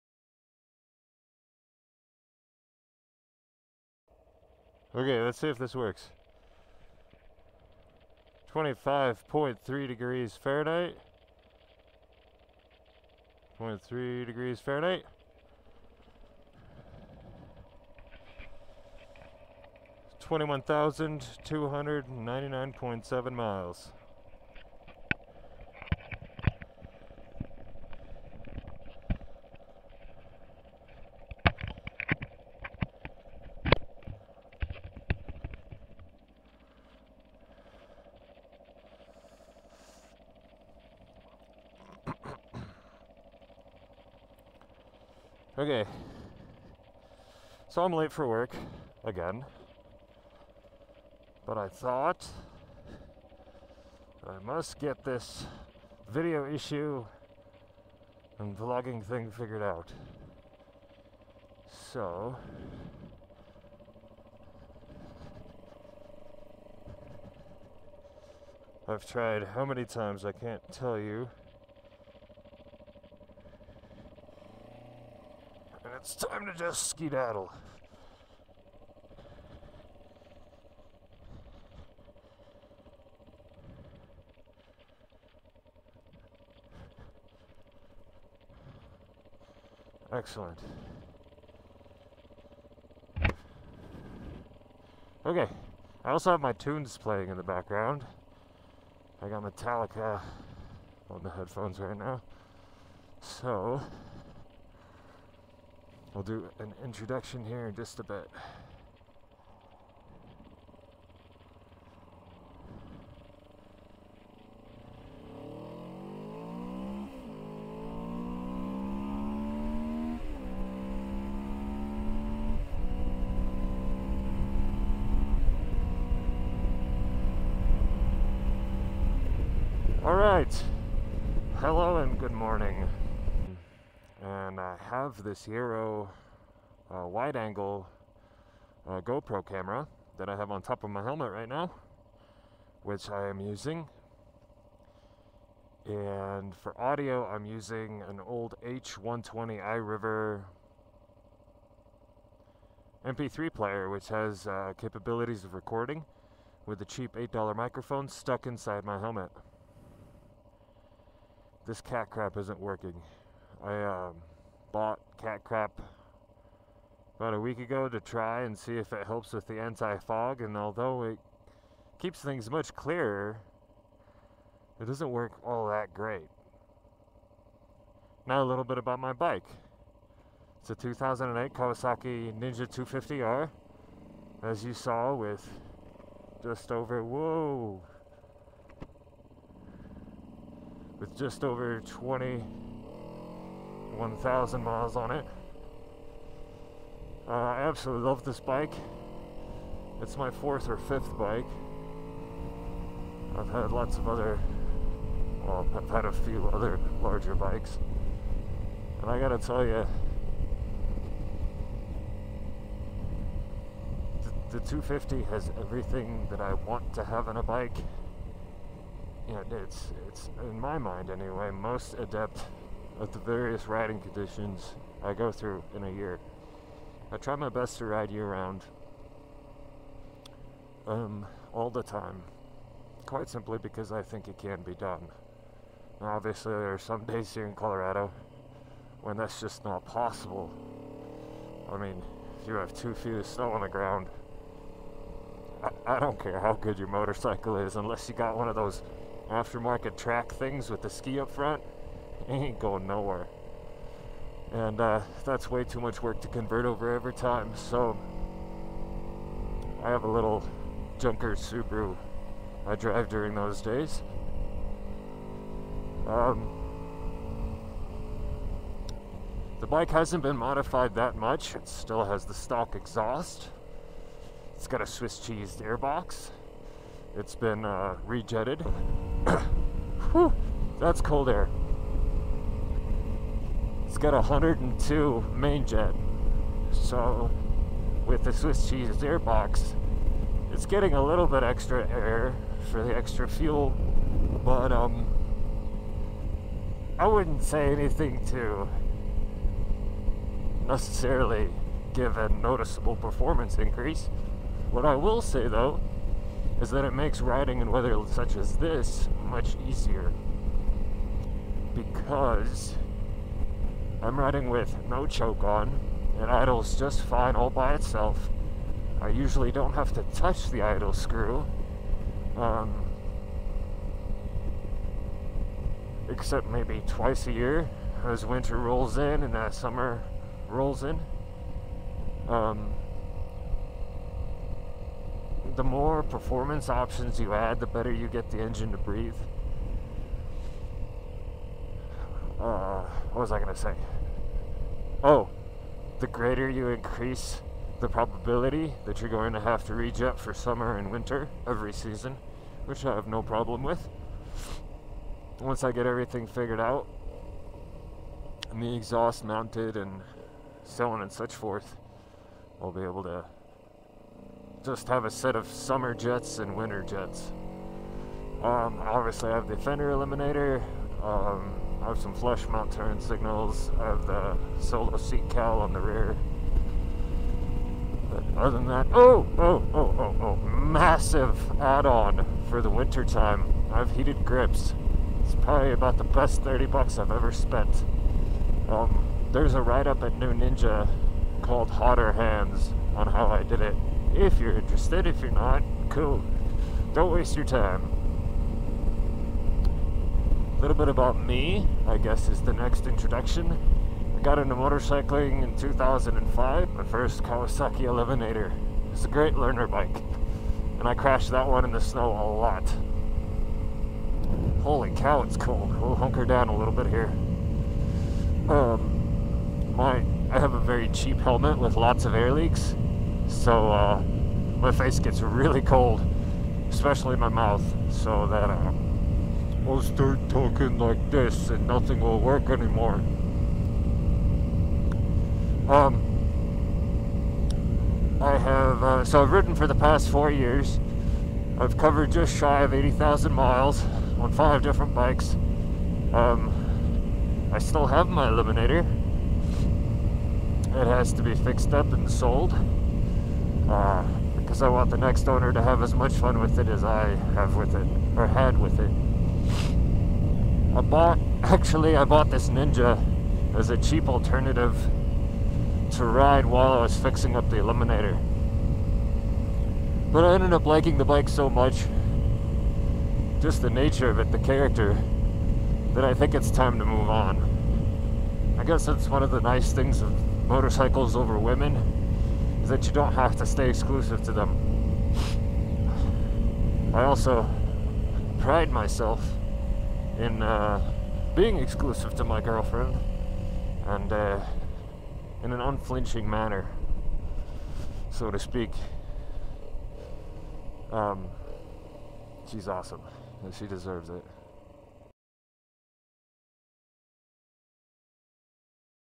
Polar Vlog 1 Part 1 on a Kawasaki Ninja 250R
To those of you who have already commented, thank you very much, and you don't have to comment again. I'm still trying to figure out my uploading issues. The audio is louder this time around, and the quality is what it is, I can't get it better, and be able to upload the file, for now.

This is a fairly boring video riding wise since it's a vlog. I introduce myself and my bike. If there are other topics you folks would like to see me cover, please comment to your heart's content on what you'd like me to speak on. I'm not necessarily an authority on anything, but I'd be happy to offer my opinion on something. Enjoy!